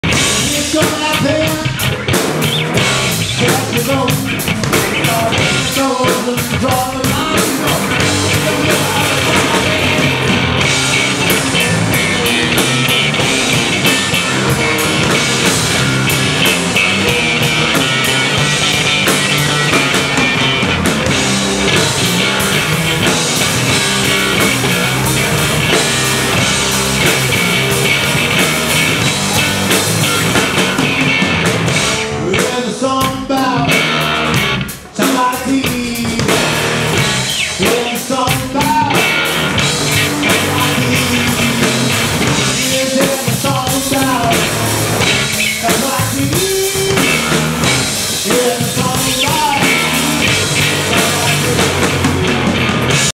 Heavy blues rocker, 6 string slide electric banjo style
Duncan and The Dragonslayers live at The Canyon in Agoura banjo #6 string